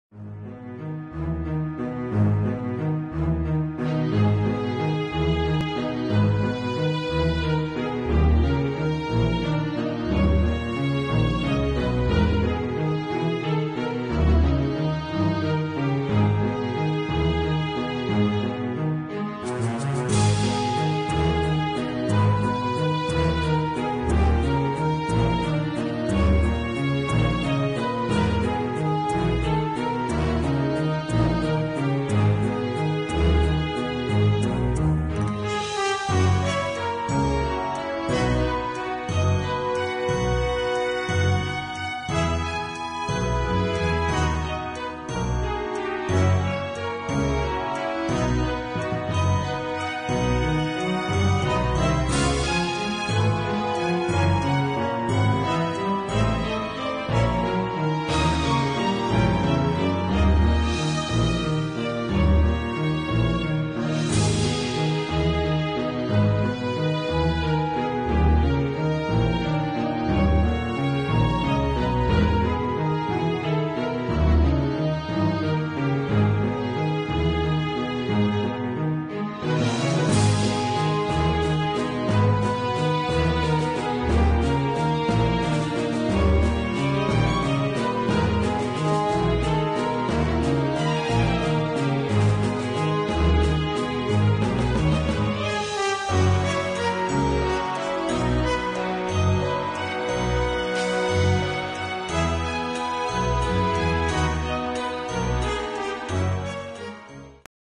The Art Of Walk by UIN Painting Shoe for Traveller.
Ultra light travel shoe by UIN for those looking for high quality light and comfortable shoe. Suitable for traveller and at any casual event.